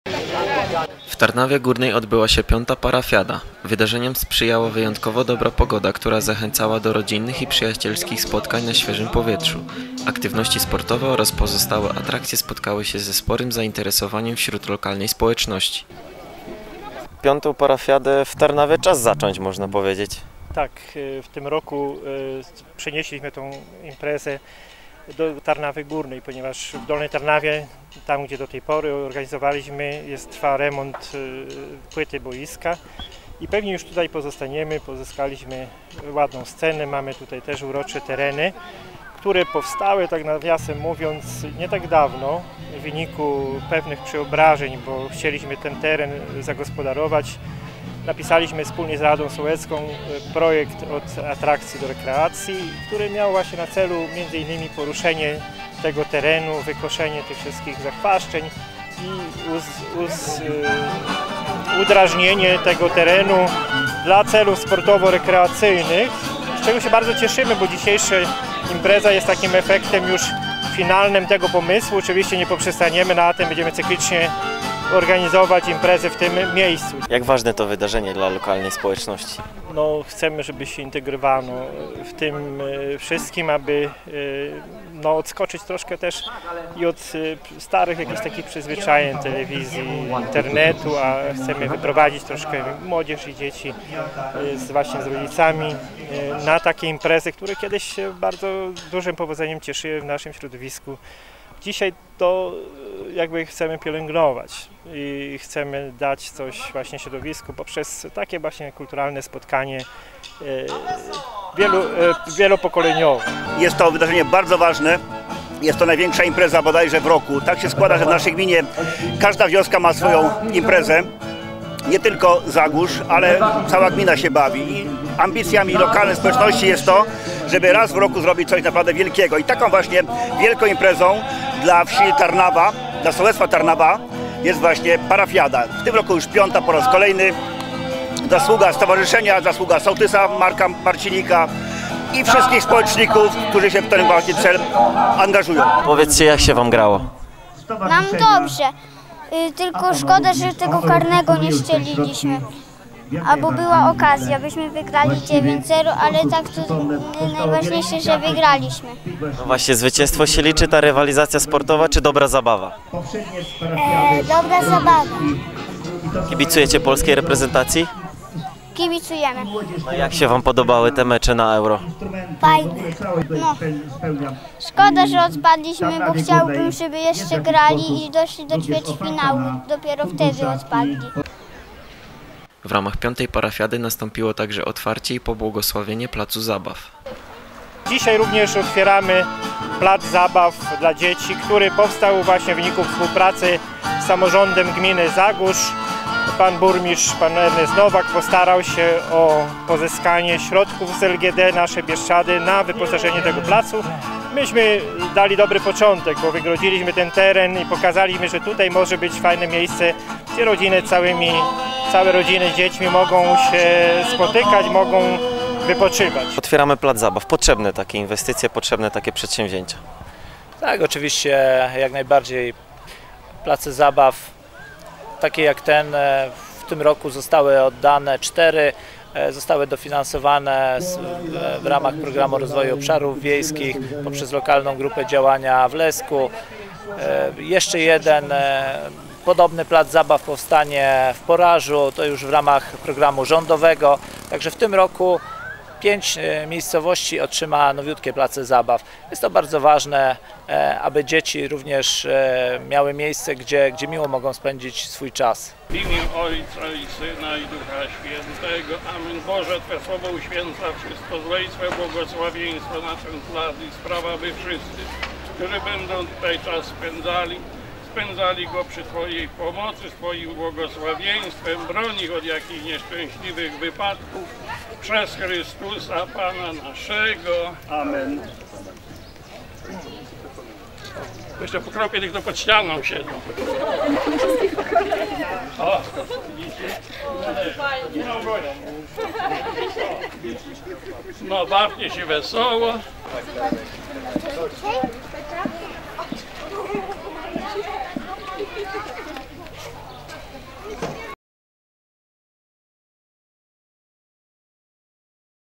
V Parafiada w Tarnawie Górnej (VIDEO HD)
TARNAWA GÓRNA/GMINA ZAGÓRZ. W Tarnawie Górnej odbyła się V Parafiada. Rozgrywkom sportowym, a także wszelkim innym atrakcjom sprzyjała słoneczna pogoda. Dopisali uczestnicy, którzy w rodzinnej atmosferze spędzili niedzielne popołudnie i wieczór.